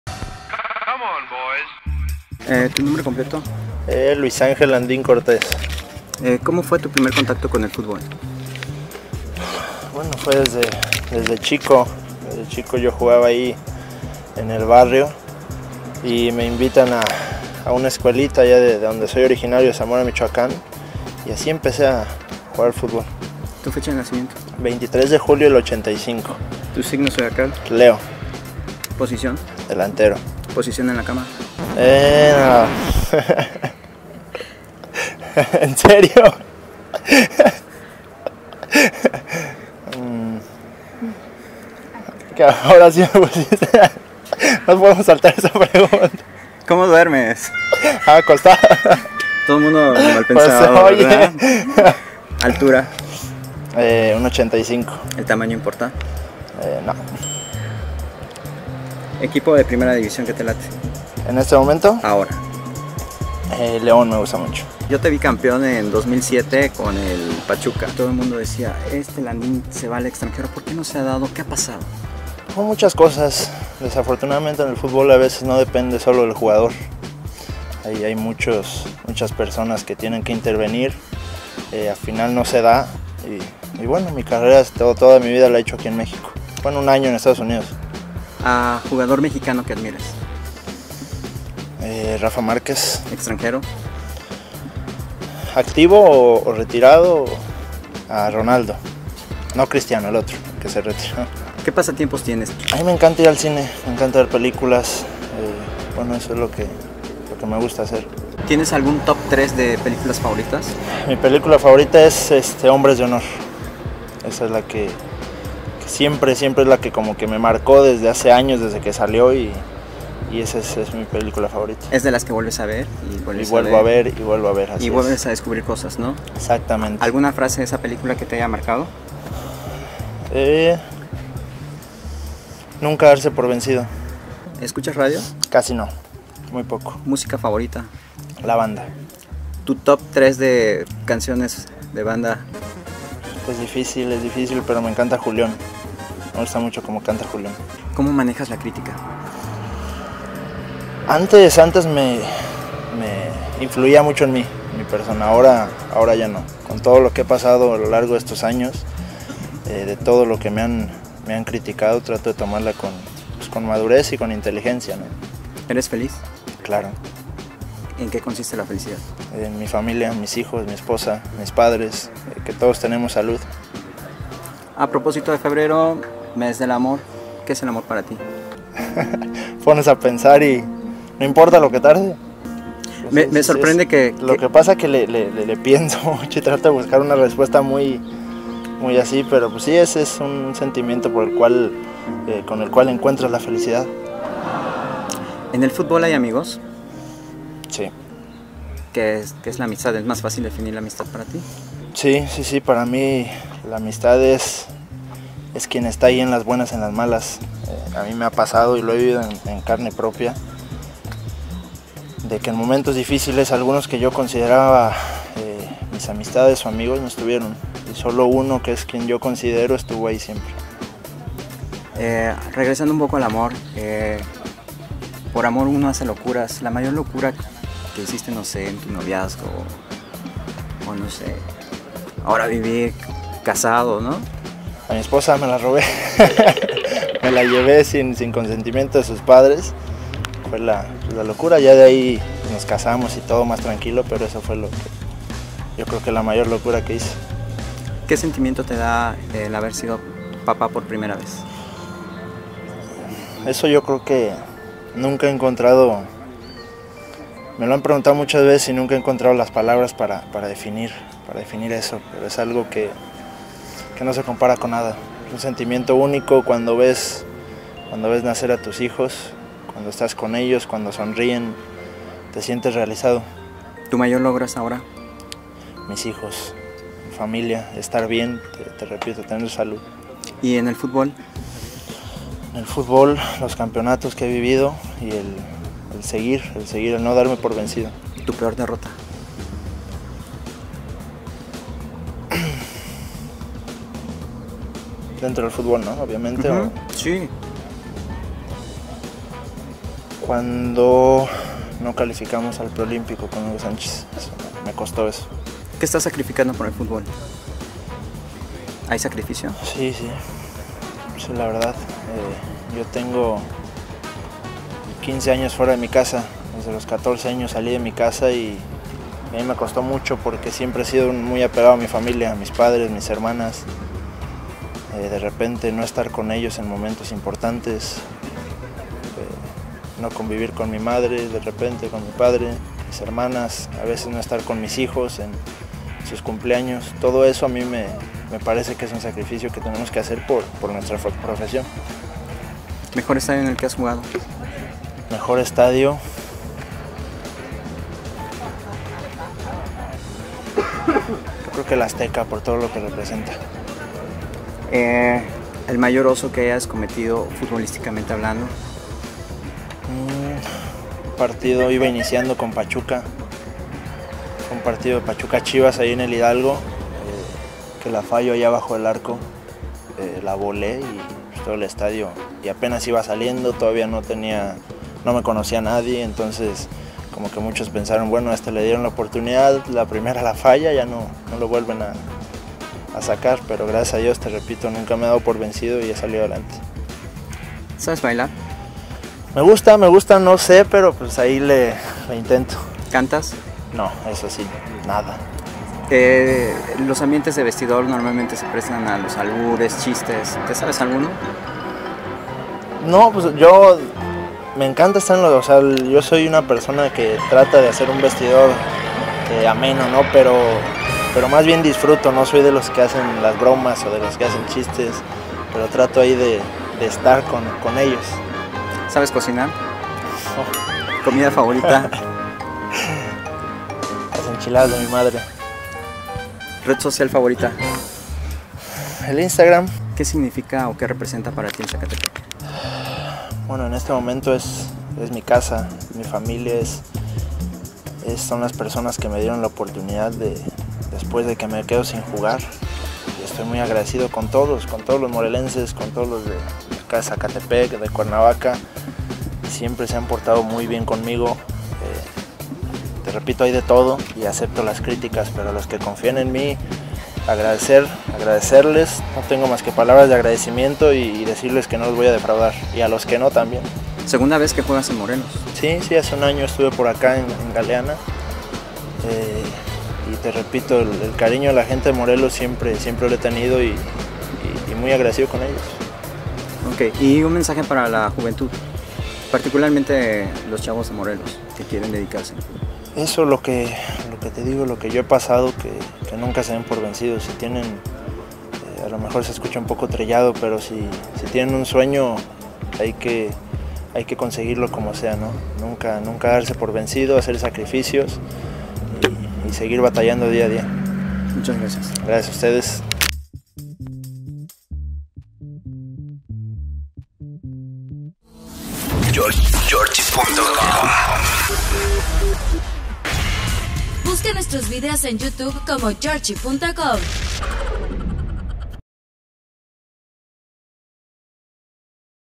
🔴tbt Luis Ángel Landín | Entrevista 🎙️| Zacatepec📸
tbt entrevista con Luis Ángel Landín para la sección de ZACAndo los trapos con Jorchy